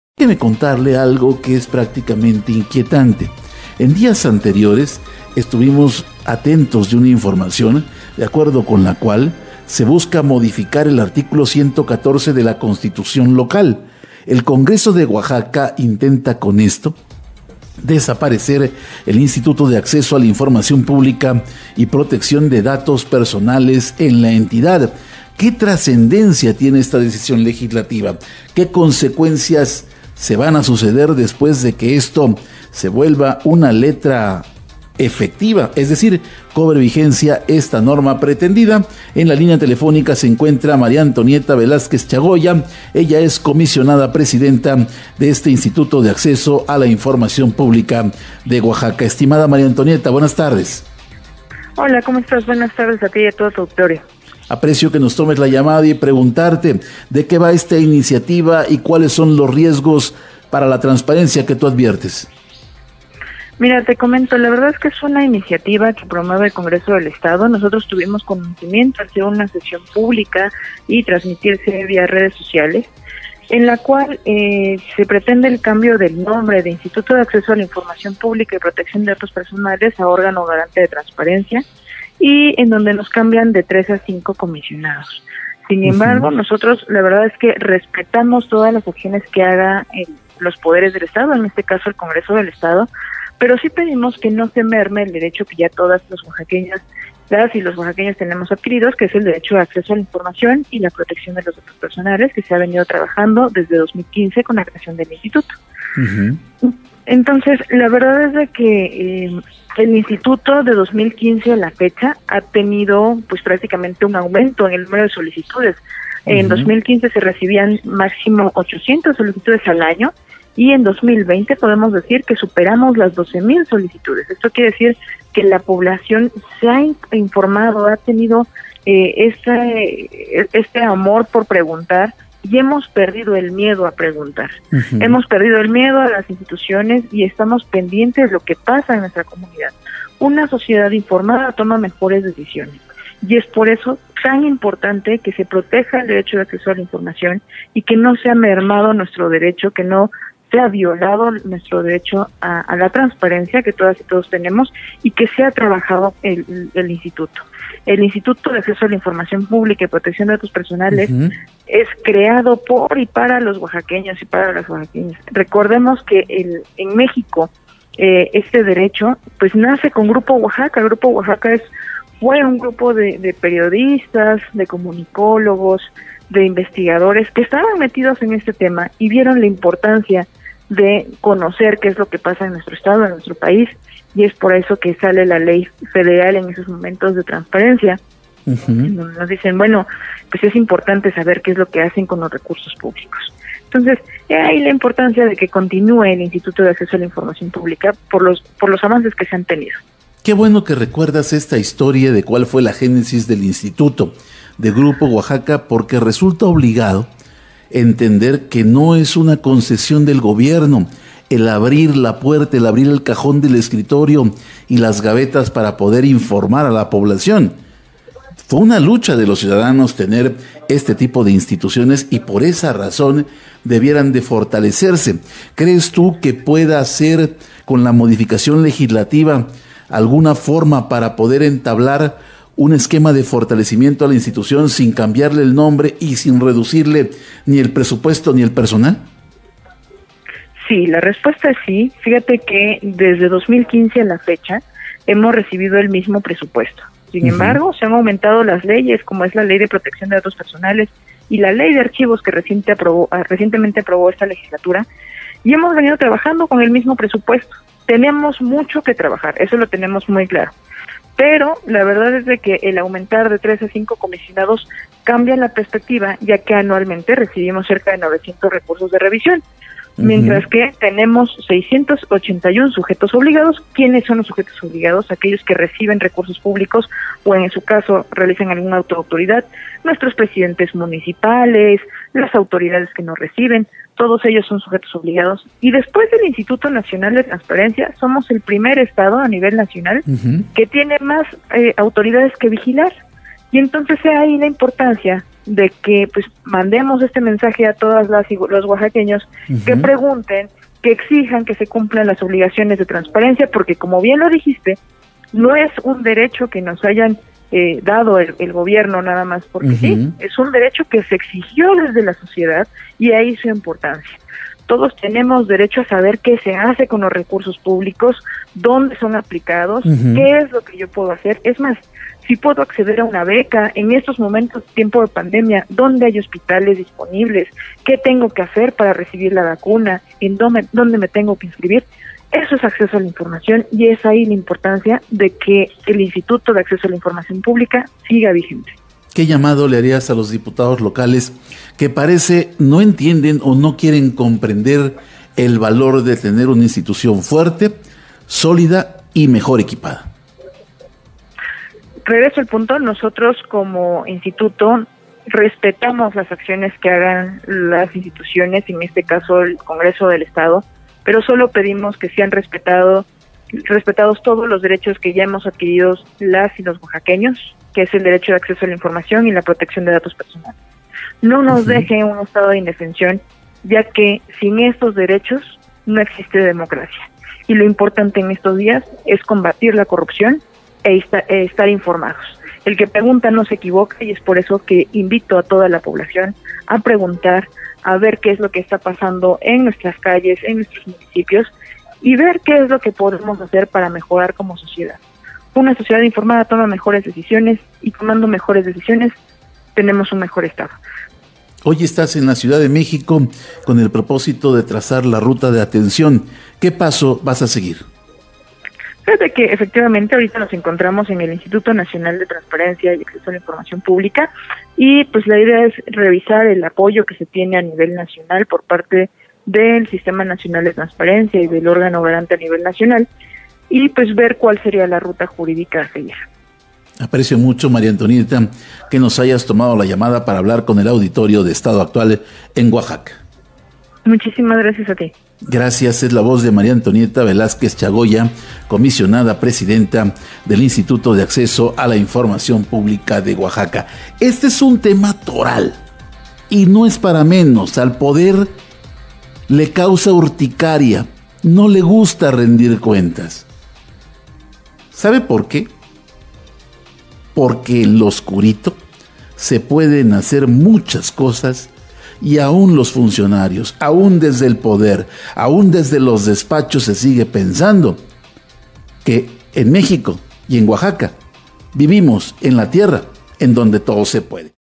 El Congreso de Oaxaca intenta desaparecer el Instituto de Acceso a la Información Pública del Estado
Con modificación al artículo 114 de la Constitución Local, el Congreso de Oaxaca intenta desaparecer el Instituto de Acceso a la Información Pública y Protección de Datos Personales de Oaxaca.